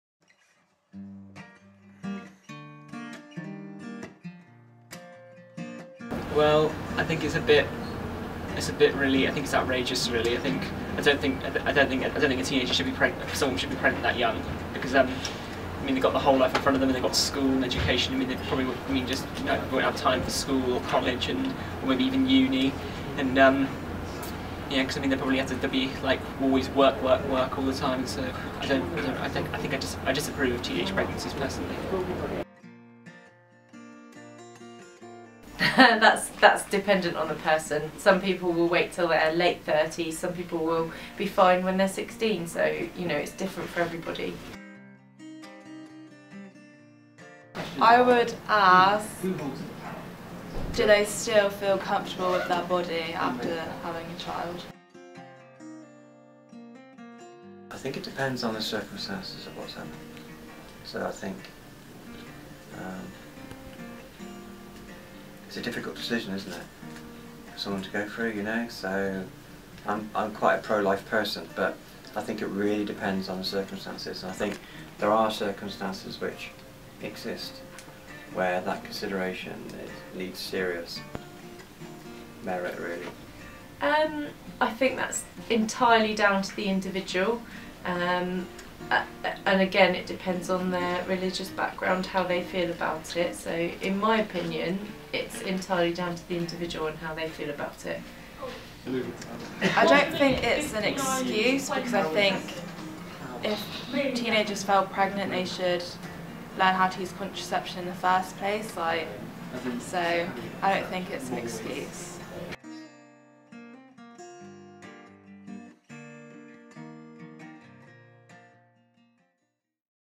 Teenage Pregnancy Documentary
college work
short (2 minute) documentary on people's views of teen pregnancy, plus a couple of little facts for you :)
camera: Abi Cannings (me)
Editing: Abi Cannings (me)
Starring: Rob, Katie, Sean and some other random woman.. we didn't ask what her name was.. oops:')